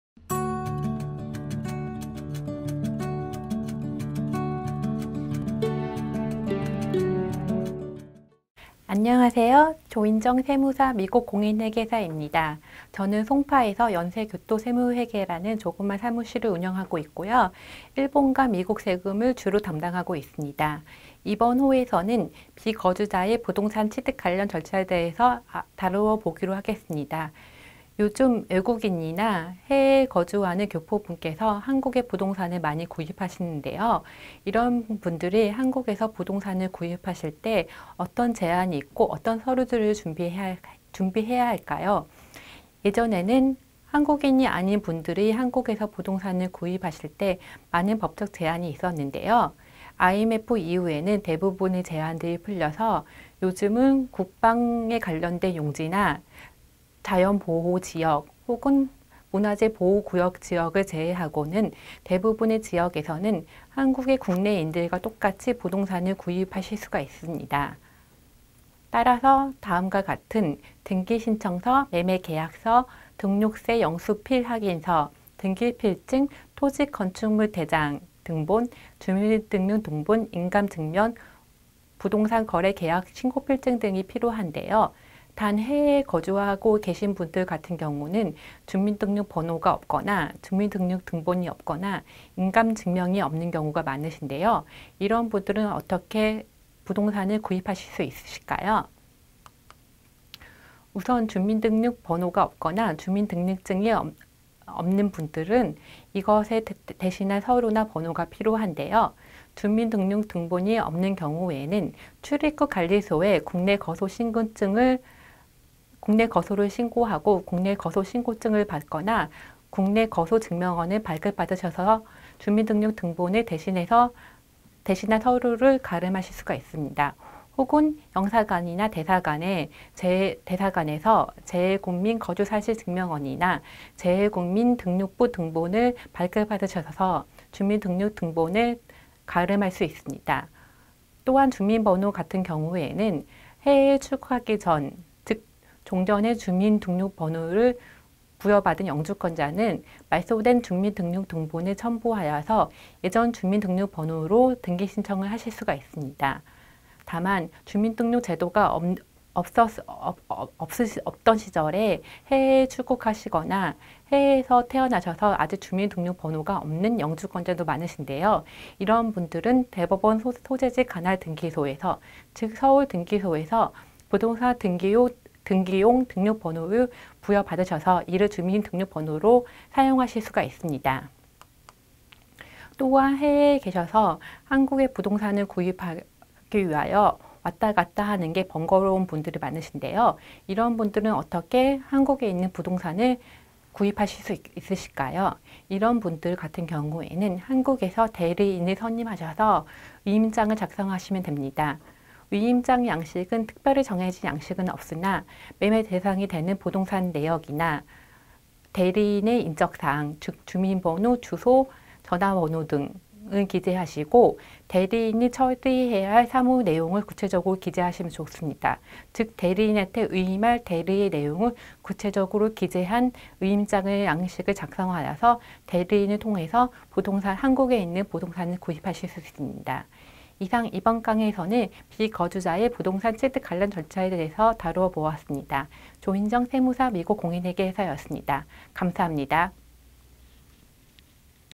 #6 [국제조세 5분특강] 비거주자의 부동산 취득 관련 절차(1) [조인정 세무사의 5분특강]
촬영시기 : 2018.10월